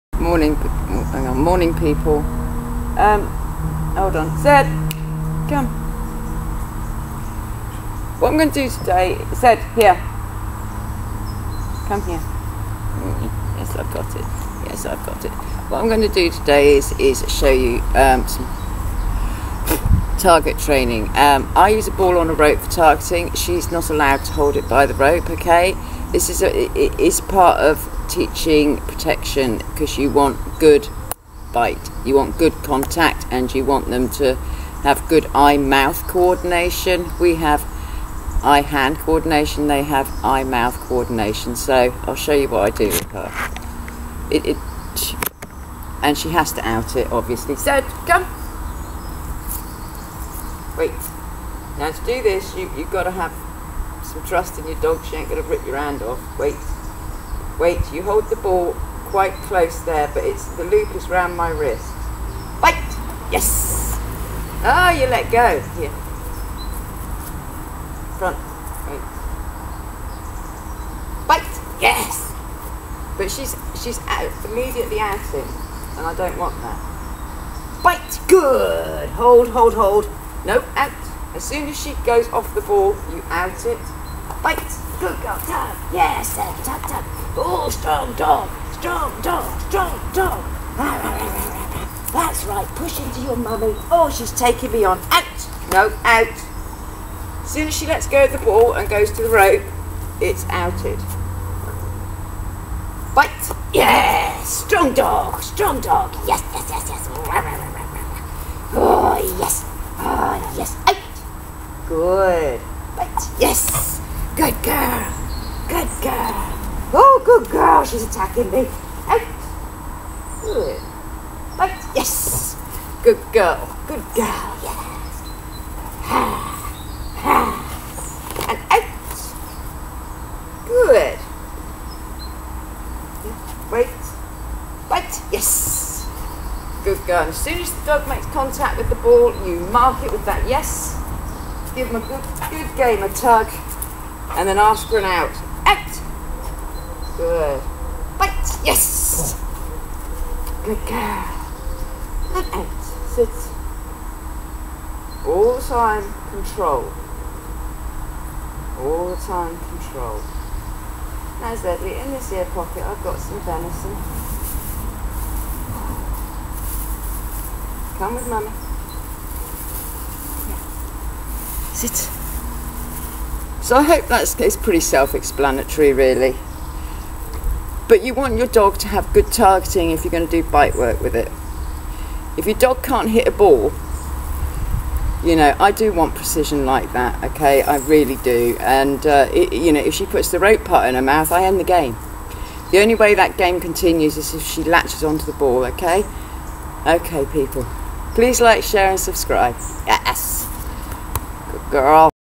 Use a Ball on a Rope to teach good Targeting
Here we show how to play a structured game of Tug plus teaching the OUT. I will only continue the game provided Zed hits the Ball. Not the rope.